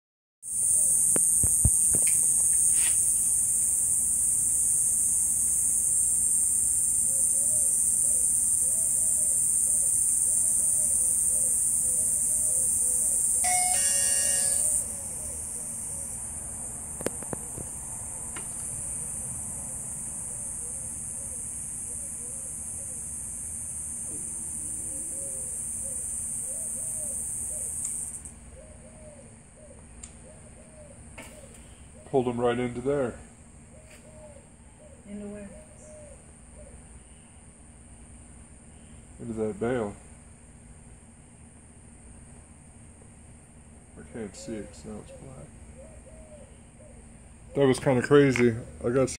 Black Widow finds a lizard
Lunch time for the Black Widow